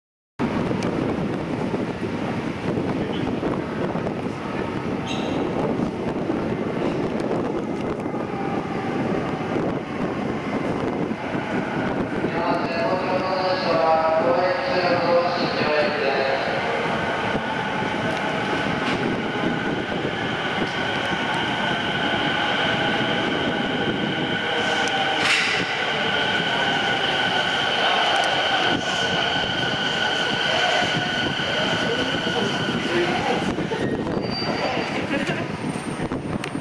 【フルHD】東京臨海高速鉄道りんかい線70-000系 東京テレポート(R04)駅停車 1
70-070F。
回送ですが、東京テレポート始発の新木場行きです。